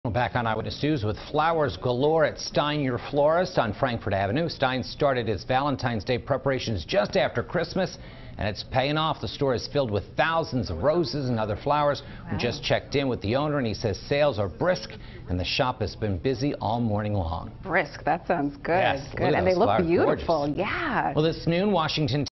Busy Valentine's Day Morning At Stein Your Florist On Frankford Avenue
Stein's started its Valentine's Day preparations just after Christmas and it's paying off.